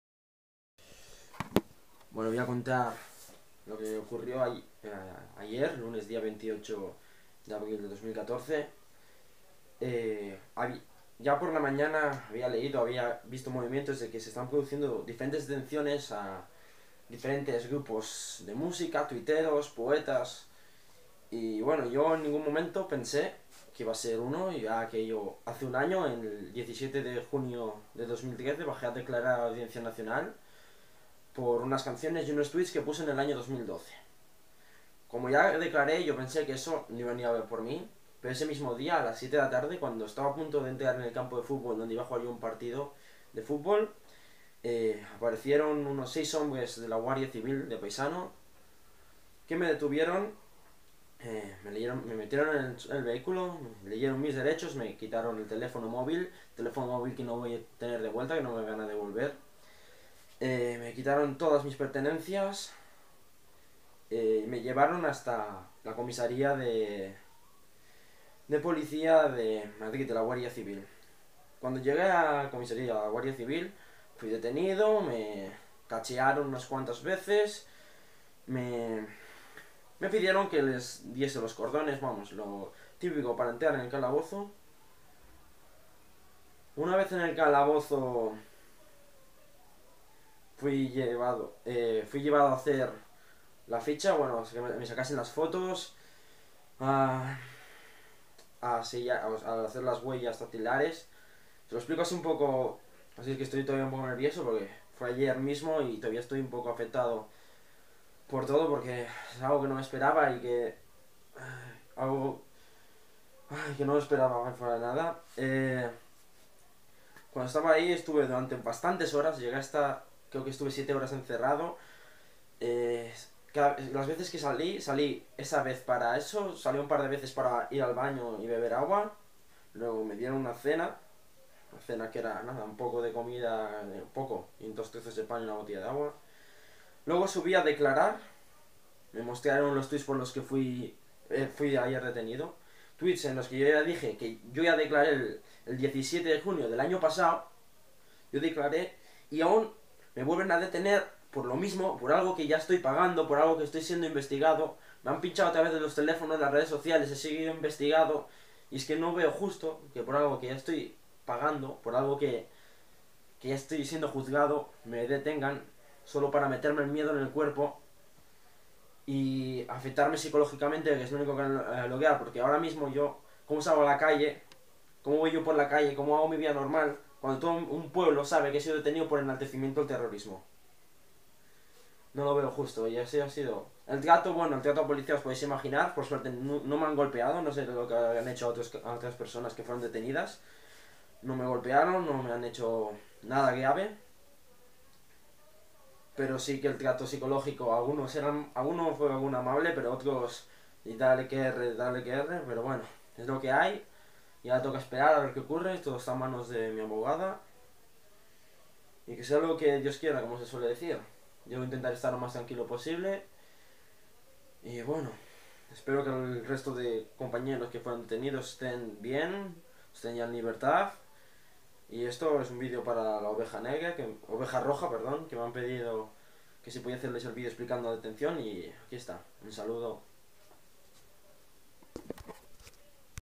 Comunicado de Ivan Riera tras su detención - La Oveja Roja
Tras mas de un año siendo vigilado con todo pinchado he sido detenido por unos tweets y canciones por las que ya declare hace un año ante la fiscalía de menores de la audiencia nacional.
Espero que el resto de compañeros estén bien, les sea leve, y estén en libertad.
Nos detienen porque nos temen.
Salud.
Iván Riera